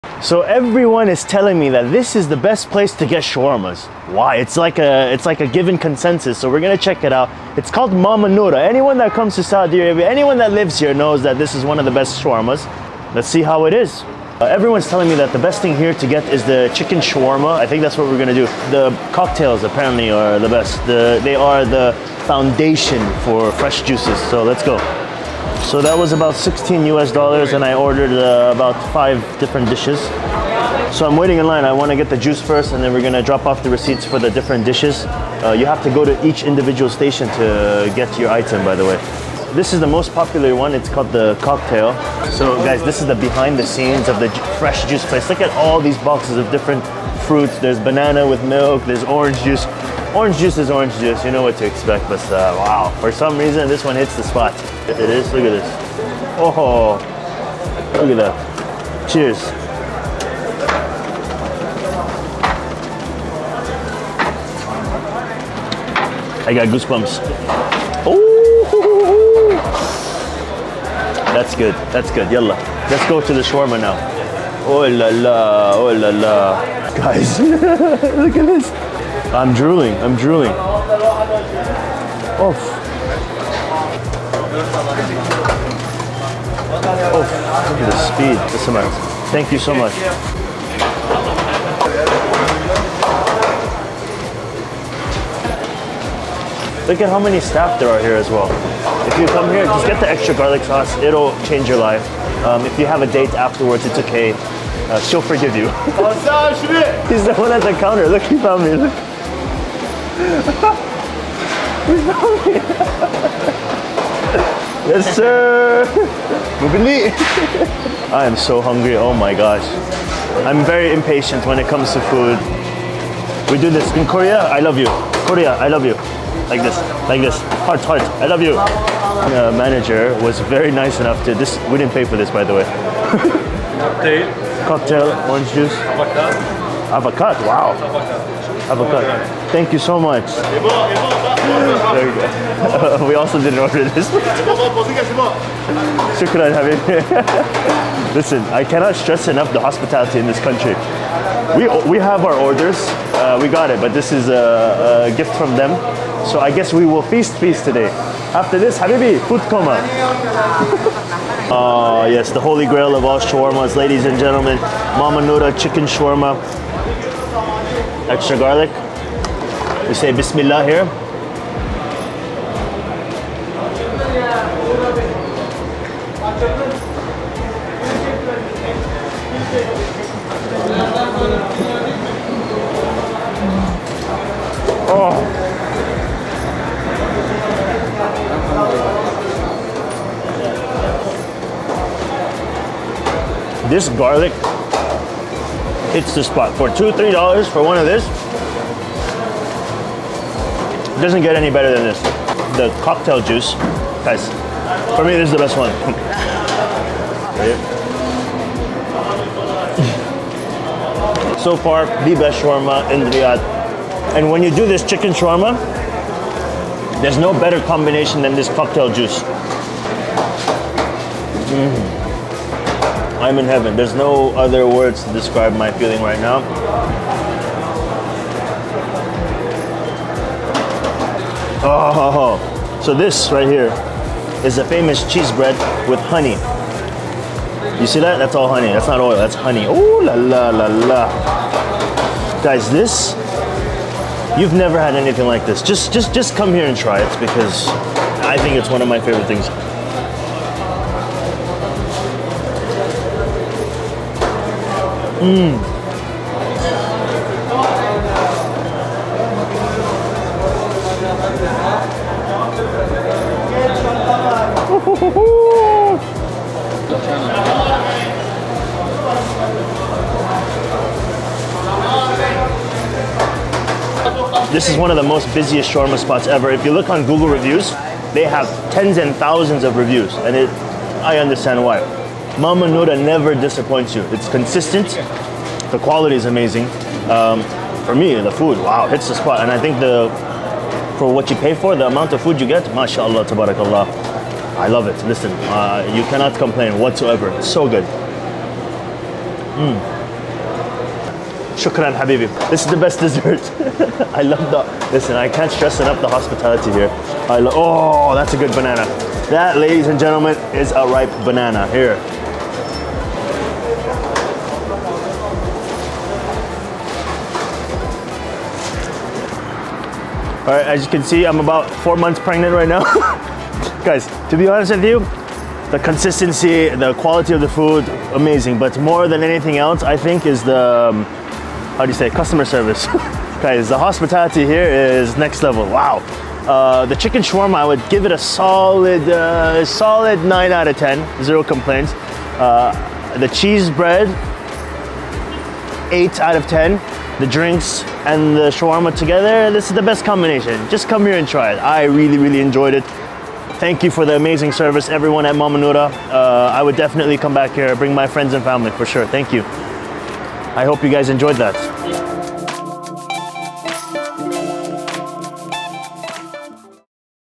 Is Mama Noura Really The Best Chicken Shawarma In Saudi Arabia?!
Riyadh, Saudi Arabia - Let me take you on a journey to arguably one of the most popular restaurants in Riyadh, Saudi Arabia. Shawarma is a dish that is made of usually chicken, home made bread with lifechanging garlic sauce and pickles. It is sold in the thousands on a daily basis in the Kingdom and is extremely popular throughout the Middle East.

When I first came to Saudi Arabia, Mama Noura was one of the first places that my friends took me to, telling me that this place is a childhood memory and a must eat as a foreigner as it is their favorite place. However, the longer I stayed in Riyadh, the more I realized that there are different shawarma spots on almost every corner you go to. So as a foodie, I made it my mission to find out for myself and tell you… “Where is the BEST Shawarma in Riyadh?”